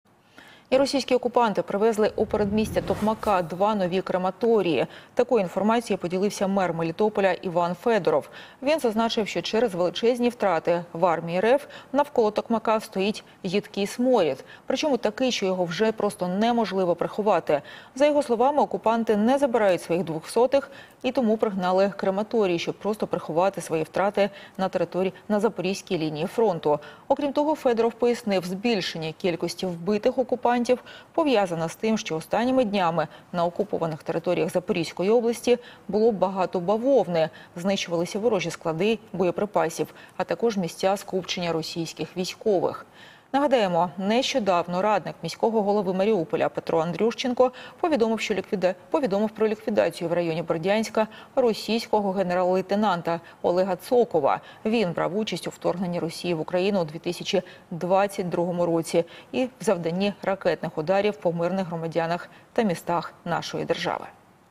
"Сморід страшенний": в окупований Токмак росіяни привезли два мобільних крематорія
Російські окупанти намагаються приховати втрати, тож до передмістя окупованого міста Токмак на Запоріжжі привезли 2 новеньких крематорії. Про це повідомив міський голова Мелітополя Іван Федоров. Він зауважив, що російські загарбники не забирають своїх "двохсотих", а лишають їх просто посеред вулиць.